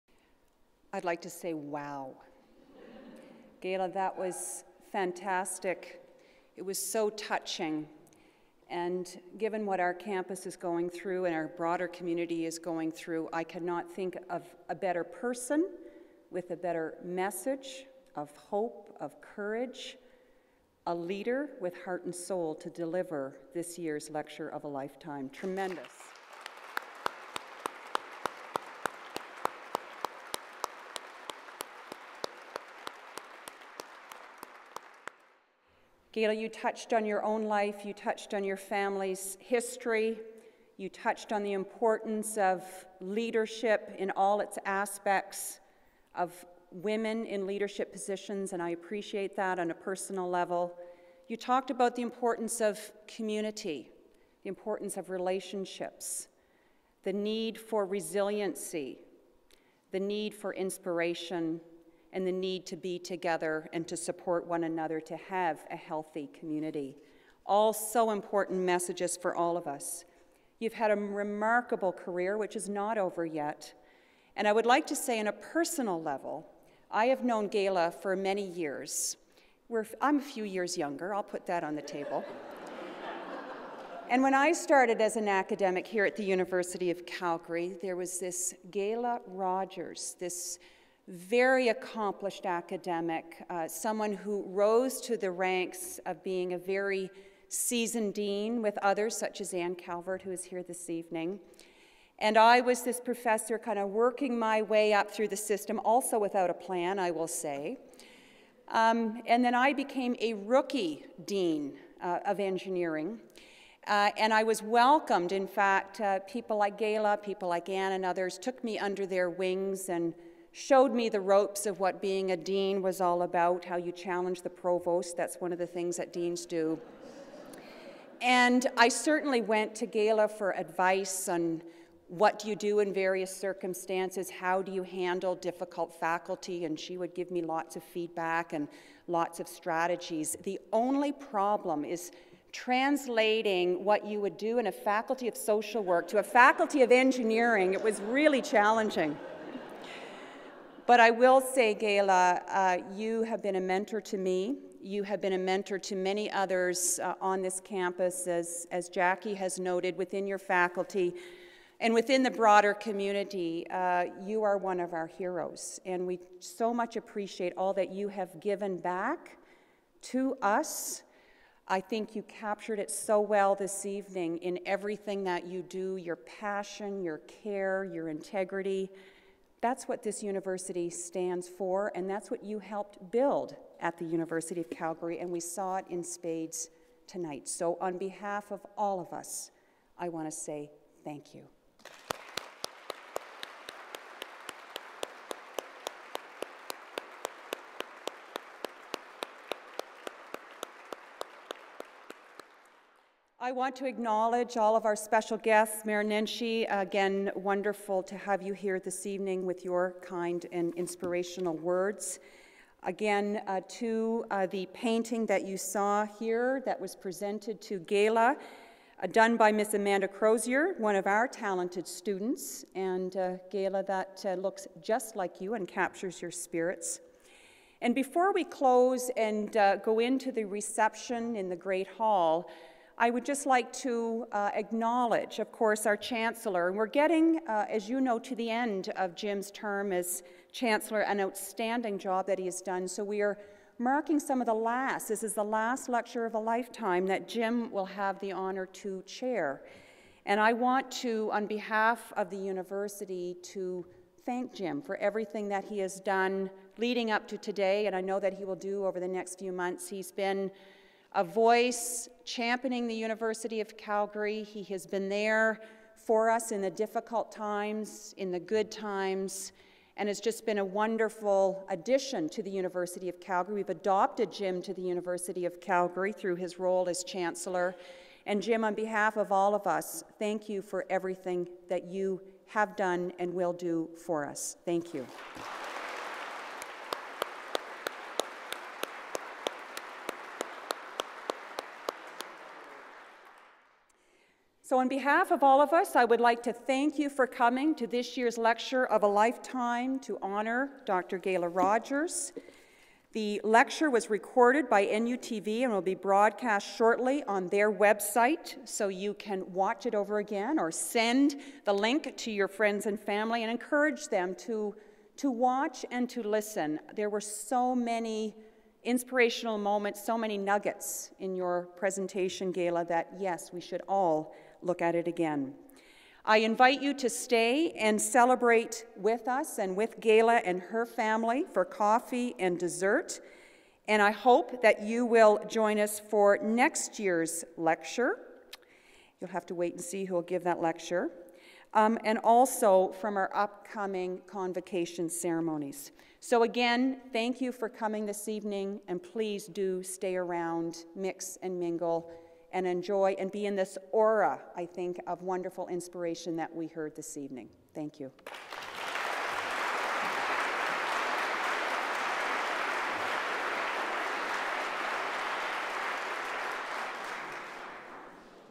UC Lecture of a Lifetime 2014 (6/6)
University of Calgary President Dr. Elizabeth Cannon concludes the event following an enthuseastic standing ovation and after presenting Dr. Rogers with a plaque in honour of the Lecture of a Lifetime and a portrait done by alumna Amanda Crozier.

Recorded live on April 16th, 2014.